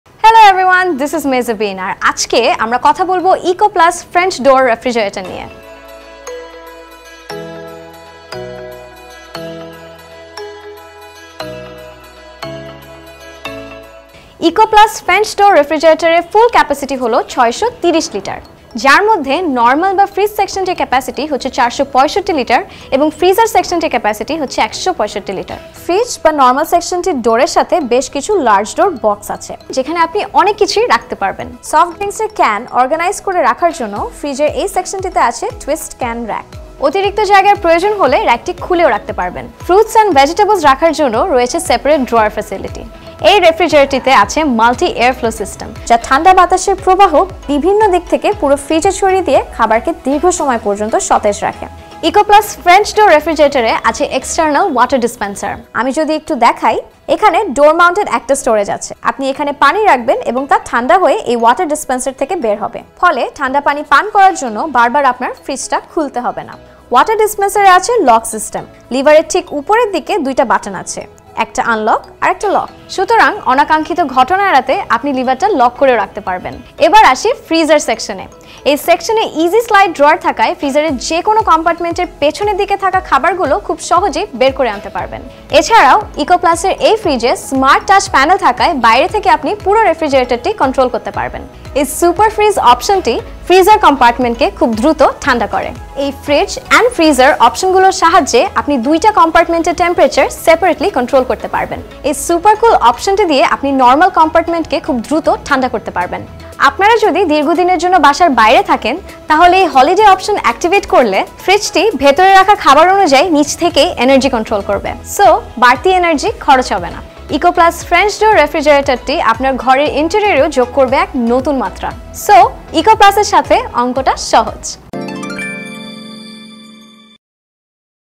Everything you need to know about the eco+ French Door Refrigerator
eco+ French Door Refrigerator features Multi Air Flow to provide equal cool air in every corner of the fridge compartment. So no food will get a chance to become stinky for the lack of cool air. Even Air distribution keeps the food compartment fresh and hygienic.

This Home Appliance provides you with more benefits and advanced features such as-

Multi Air Flow System
LED Display with Touch Button Control
External Water Dispenser
Folded coke rack
Easy-Slide Freezer Drawer
Movable twist ice-maker

eco+ French Door Refrigerator has a huge capacity of 630 liters with a large door box. The twist can rack makes it more organized and user-friendly. The separate drawer facility helps to keep fruits, and veggies separate and fresh. You can have a cold glass of water without opening the refrigerator door with the help of the external water dispensal system. This smart feature makes life more easier. The freezer section has a simple sliding drawer that makes getting food from the back easier. You can control the fridge from smart touch panel from the outside. The super fridge feature makes the freezer potion more cooler in just aminute. You can set different temperatures for the refrigerator and freezer. The smart Holiday function relieves you of the stress of giving a long tour.

Time Code
0:00 Introduction
0:18 630 Liter capacity
0:30 Large Door Box
0:37 Twist Can Rack
0:42 Separate Drawer faculty
0:47 Multi Air Flow
1:20 Easy Slide Drawer
1:28 Smart Touch Panel
1:32 Super Fridge Option
1:43 Super Cool Option
1:50 Holiday Option
1:57 Conclusion